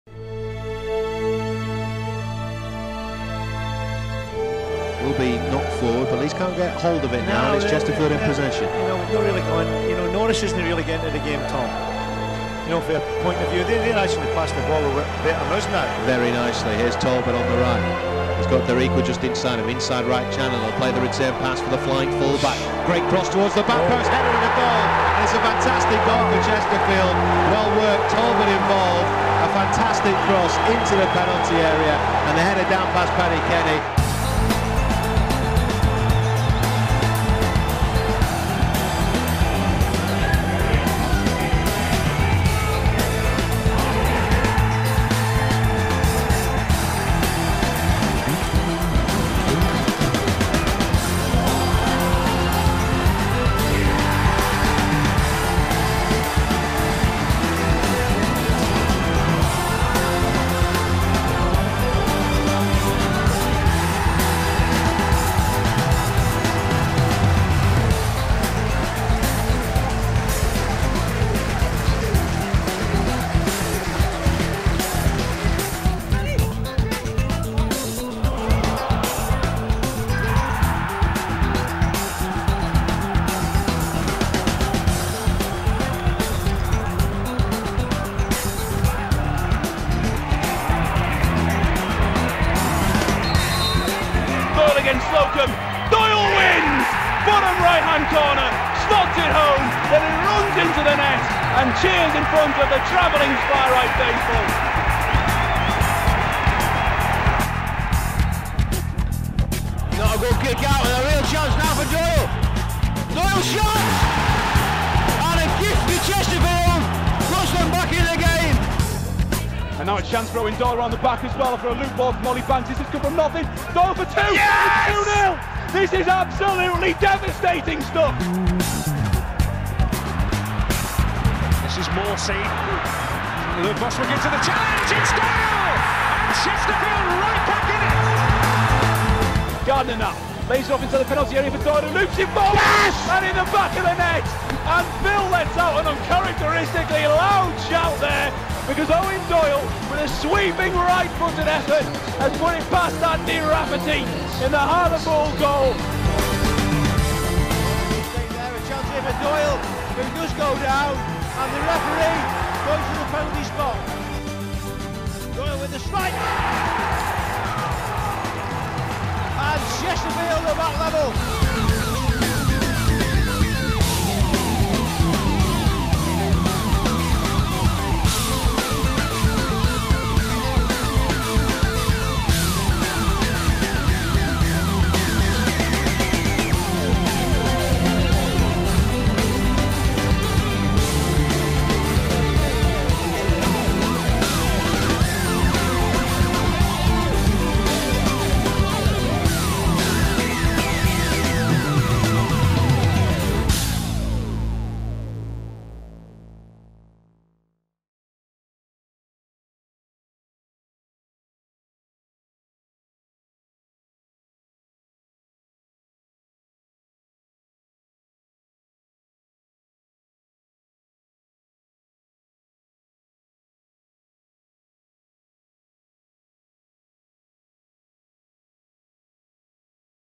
2013/14 - Top Goalscorer - Eoin Doyle
The video played to accompany Eoin Doyle's top goalscorer award at the 2013/14 Player of the Year Awards on Sunday night.